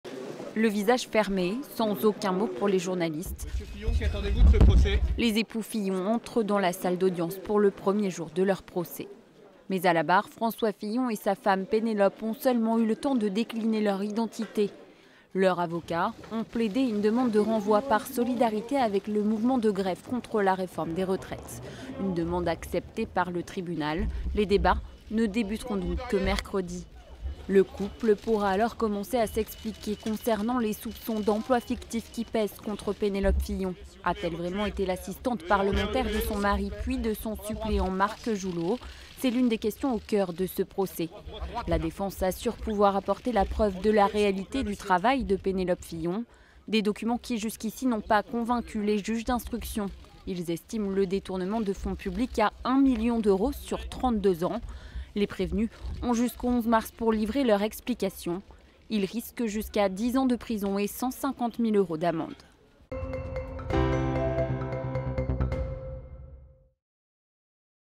Le procès Fillon renvoyé à mercredi
Le procès de l'ancien Premier ministre François Fillon, de son épouse Penelope et de son ancien suppléant Marc Joulaud pour des soupçons d'emplois fictifs de Mme Fillon, tout juste ouvert lundi, a été reporté à mercredi après-midi en raison du mouvement de grève des avocats.